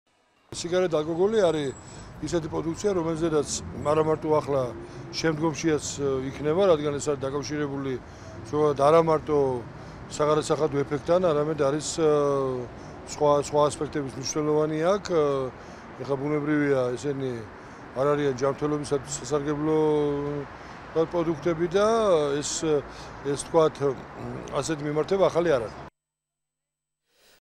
ნოდარ ებანოიძის კომენტარი სიგარეტსა და ალკოჰოლზე აქციზის განაკვეთის ზრდაზე
საქართველოს პარლამენტის საფინანსო-საბიუჯეტო კომიტეტის თავმჯდომარის პირველი მოადგილის, ნოდარ ებანოიძის კომენტარი  სიგარეტსა და ალკოჰოლზე აქციზის განაკვეთის ზრდაზე.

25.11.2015